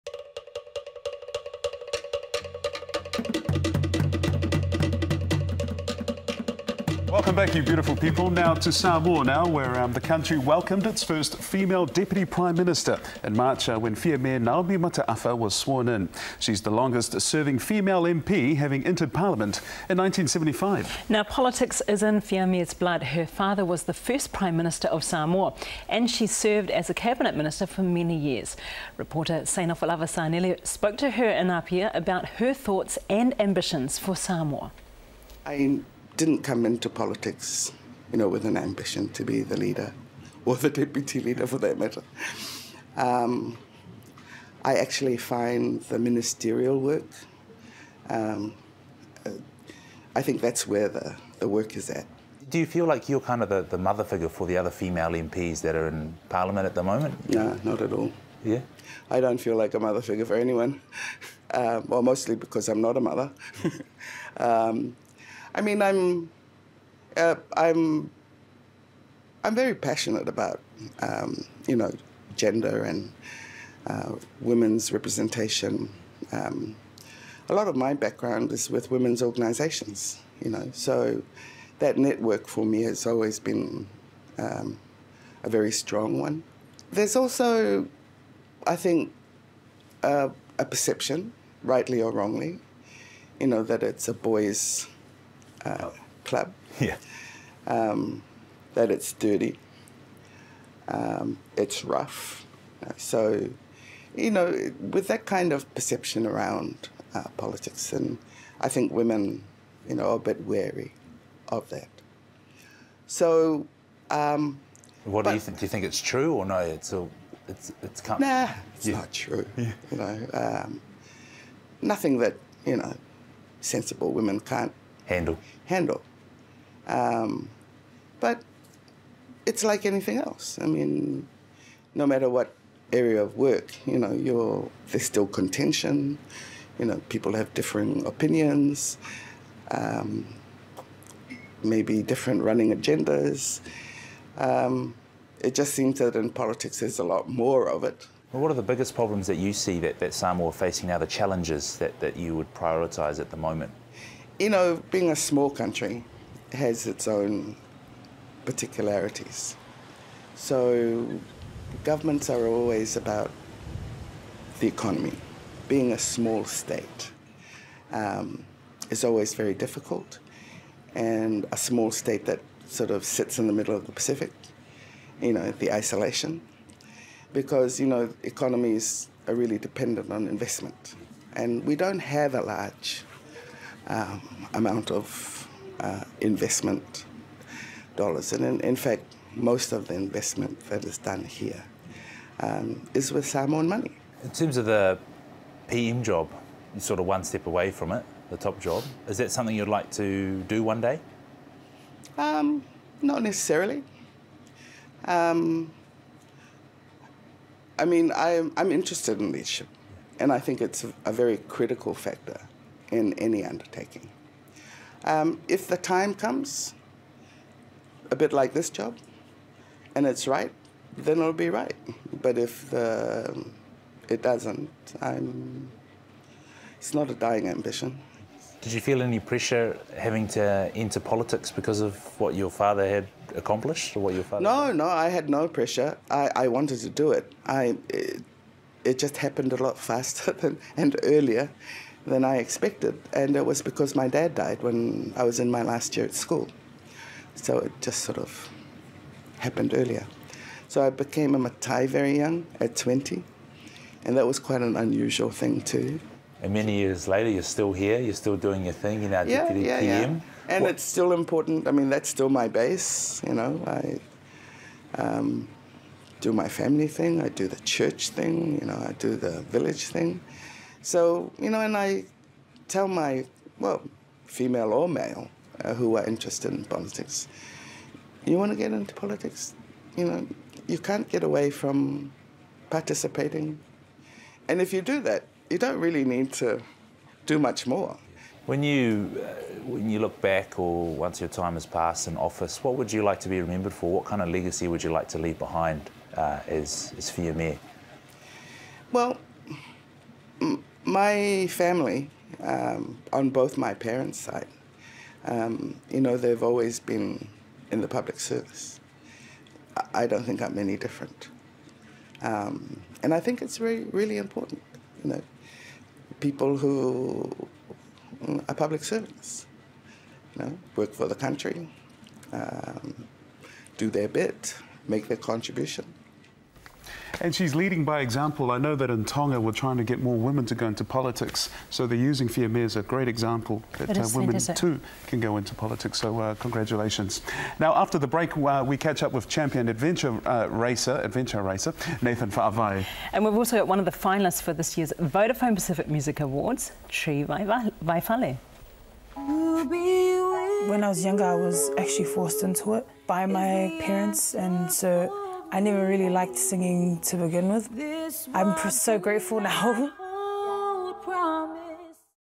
Hon. Fiame Mata'afa - Samoa Deputy Prime Minister
Samoa welcomed it's first female Deputy Prime Minister in March, when Hon. Fiame Naomi Mata'afa  was sworn in. She's the longest serving female MP having entered parliament in 1975. Reporter Seinafolava Sanele Chadwick with the story...